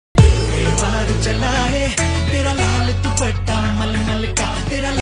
Comment “links”
𝐅𝐨𝐥𝐥𝐨𝐰 @skincarey.ing 𝐟𝐨𝐫 𝐩𝐚𝐫𝐭 𝟐

These products can be used to make your background aesthetically beautiful to shoot for products display.